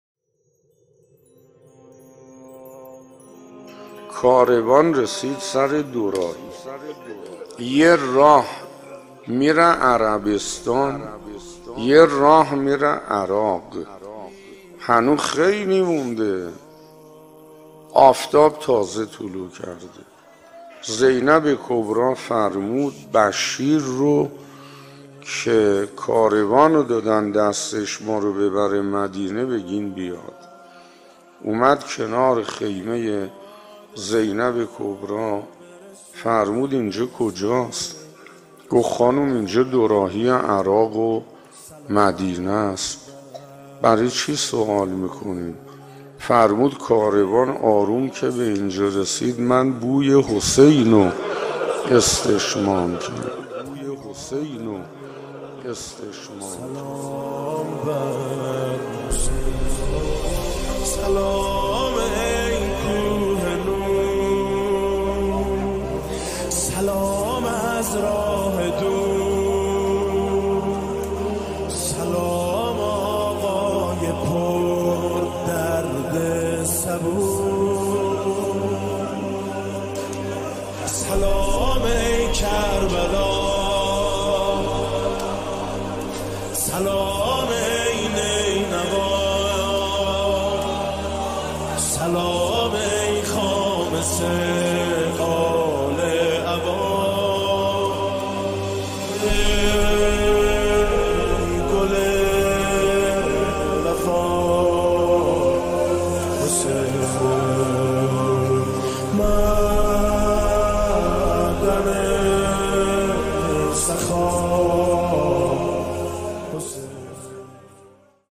بوی حسین(ع)
زینب کبری(س) و استشمام بوی ابی‌عبدالله(ع)/بمناسبت شب جمعه،شب زیارتی ابی عبدالله علیه اسلام/مدت 2 دقیقه/حجم 24 مگابایت/صفر1398-تهران،حسینیه هدایت/لینک سخنرانی